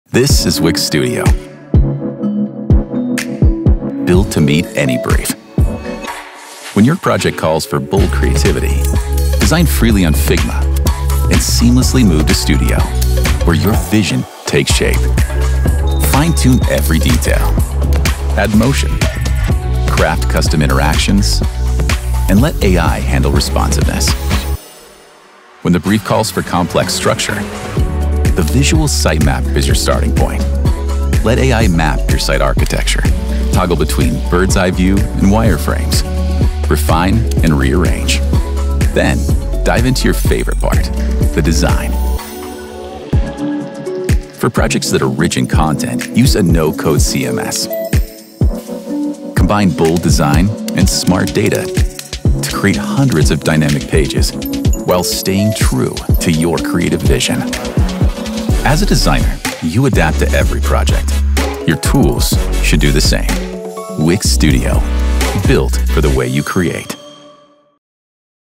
Wix Studio | Built for any brief: seamless design, structure & scale
No matter the project—bold creativity, structured planning, or scalable content—Wix Studio adapts to your process. Design freely in Figma and bring it to life in Studio, map your vision with AI-powered sitemaps, and scale effortlessly with a no-code CMS. Whatever the brief, Wix Studio gives you the tools to execute it with precision, flexibility, and creative freedom.